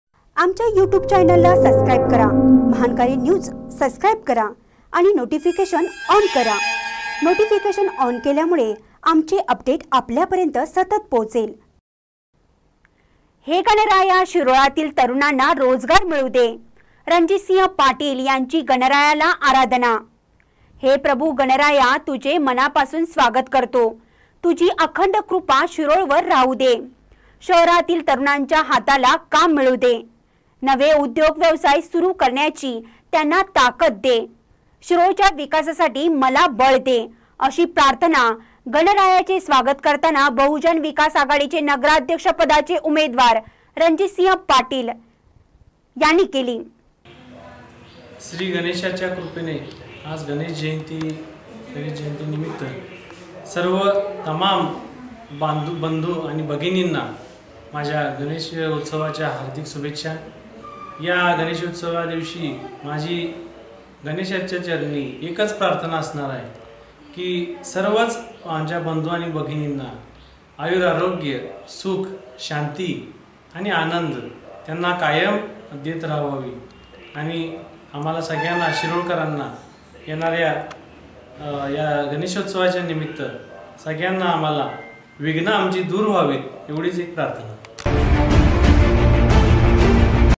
हे गणराया शिरोळातील तरुणांना रोजगार मिळु दे , रणजितसिंह पाटील
हे गणराया शिरोळातील तरुणांना रोजगार मिळु दे , रणजितसिंह पाटील यांची गणरायाला आराधना